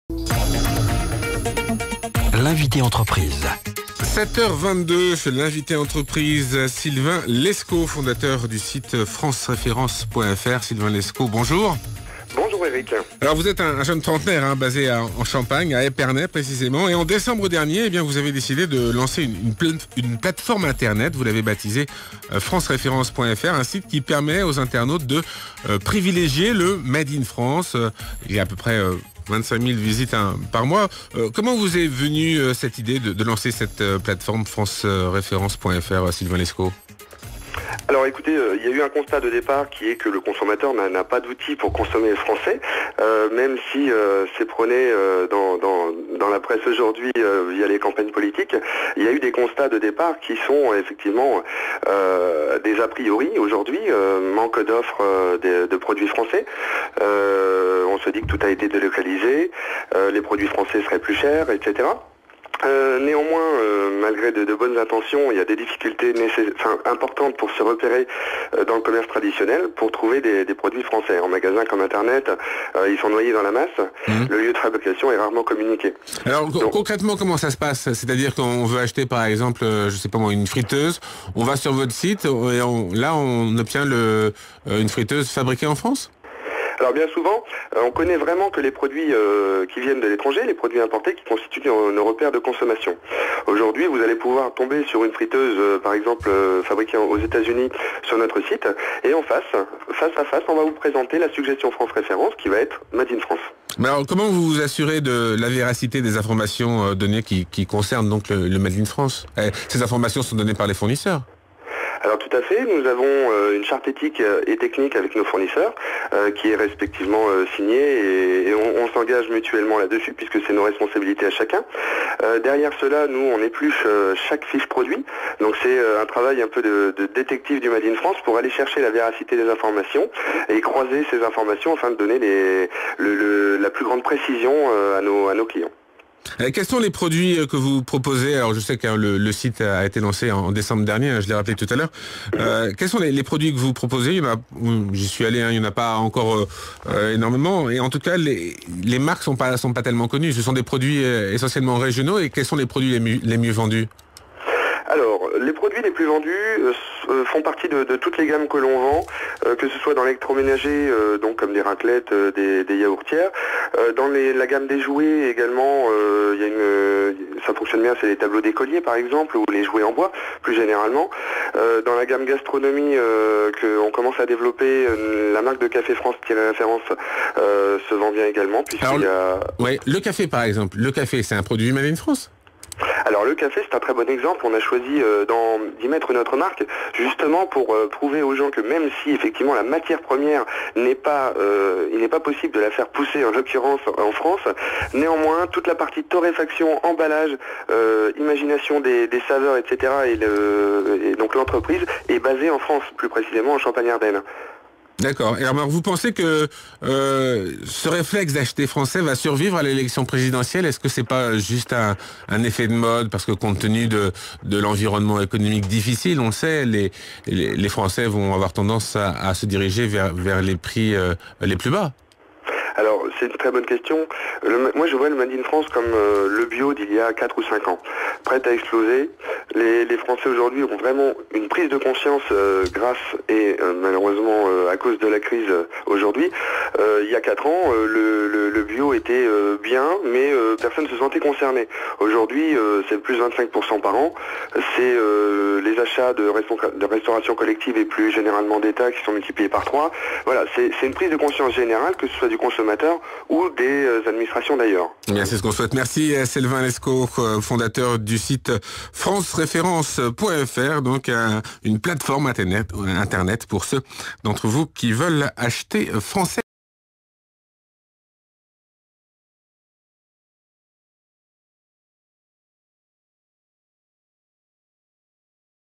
France Référence en direct sur Radio Classique
L'invité entreprise du 18 Avril 2012 : France Référence